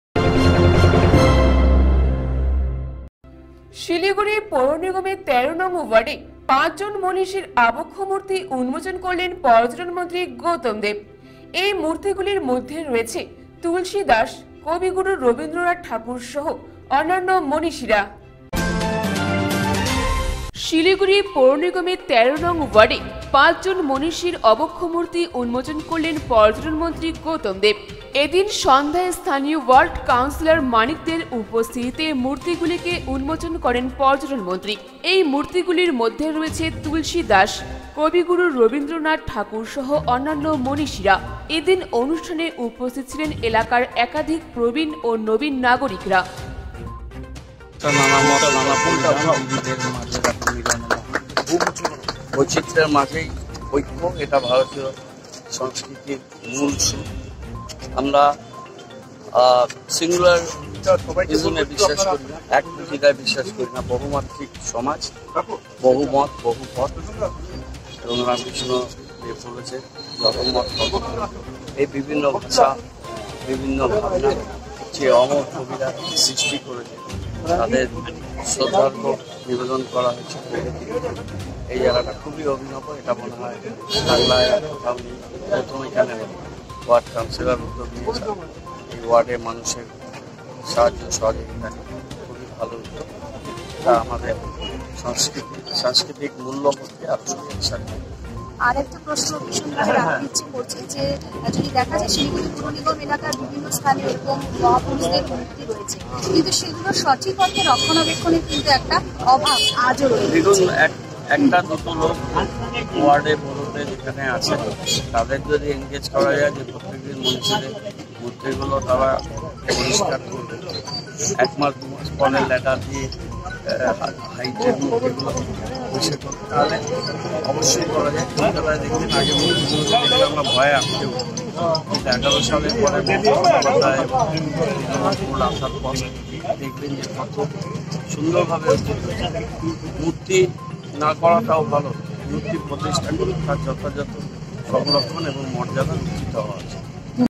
শিলিগুড়ি পুরনিগমের ১৩ নং ওয়ার্ডে ৫ জন মনীষীর অবক্ষ মূর্তি উন্মোচন করলেন গৌতম দেব
শিলিগুড়ি পুরনিগমের ১৩ নং ওয়ার্ডে ৫ জন মনীষীর অবক্ষ মূর্তি উন্মোচন করলেন গৌতম দেব,এই মূর্তিগুলির মধ্যে রয়েছে তুলসীদাস ,কবিগুরু রবীন্দ্রনাথ ঠাকুর সহ অন্যান্য মনীষীরা